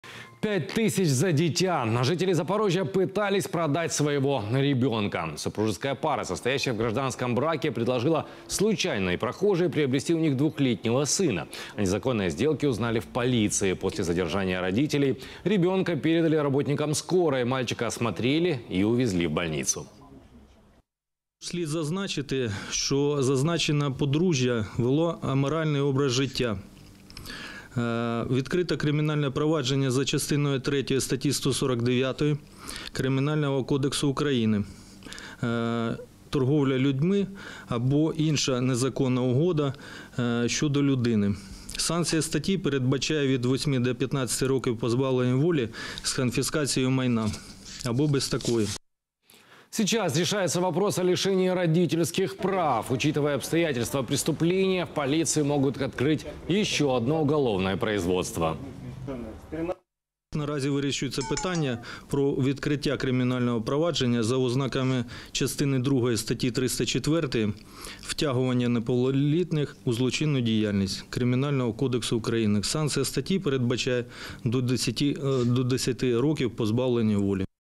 Продавала родного ребёнка, как скотину на рынке
Запорожье и всю Украину всколыхнула резонансная история, произошедшая в нашем городе. 29-летняя запорожанка решила выторговать за своего двухлетнего сына ни много, ни мало – 5 тысяч долларов.